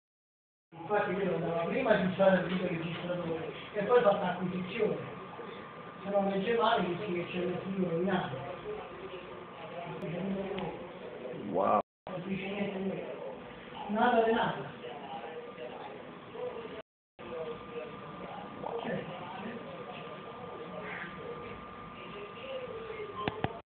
Inside Betacam
Come funziona un lettore di nastri betacam SP?